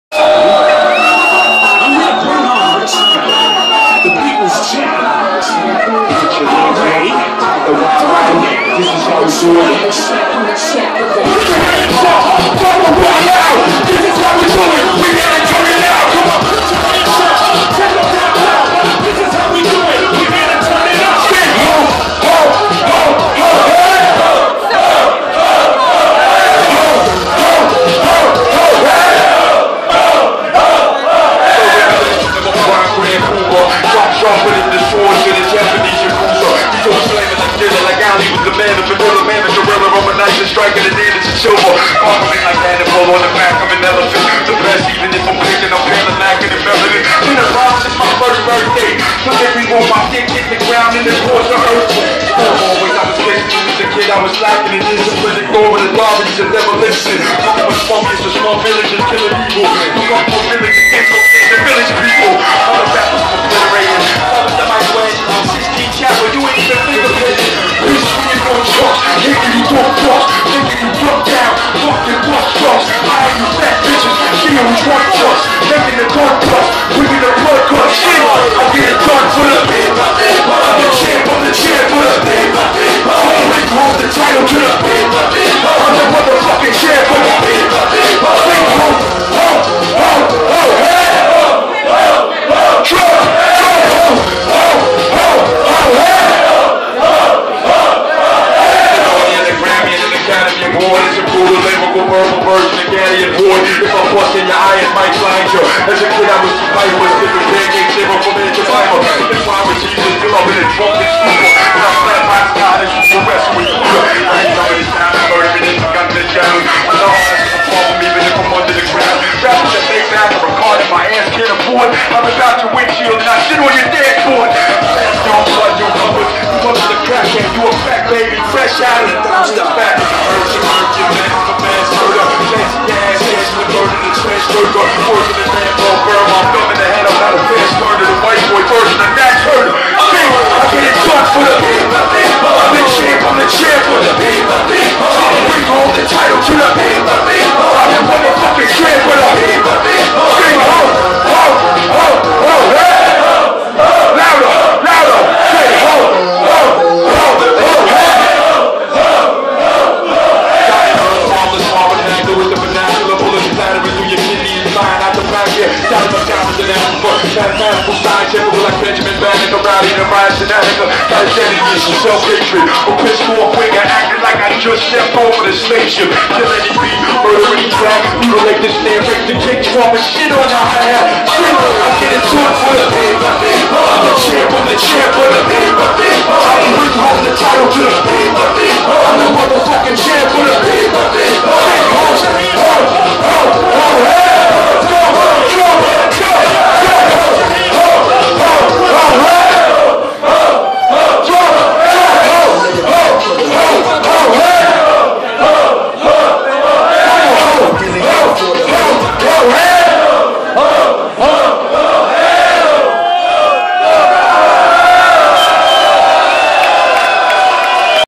R.A. The Rugged Man 4/18 live HD @ Roma 28-12-13
R.A. The Rugged Man direttamente da New York City a Roma al Rashomon il 29 Dicembre 2013, organizzato da Rugin'Bull Mgmt.

Facebook: R.A. The Rugged Man Official Page

Nino
PALERMO CITY